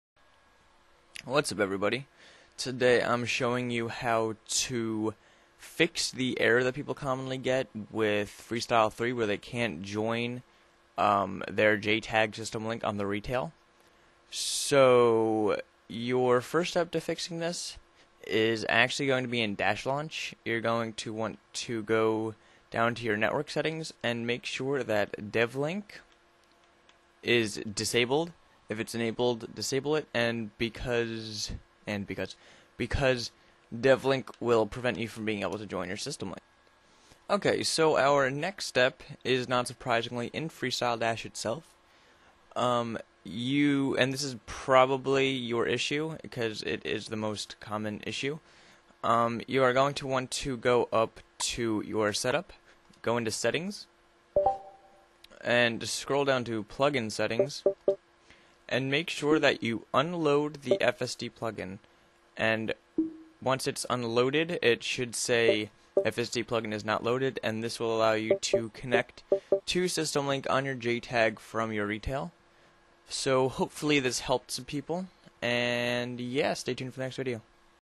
{How To} Fix FSD system link error
How to fix the error with freestyle dash where you can't connect to your jtag's sytem link on your retail.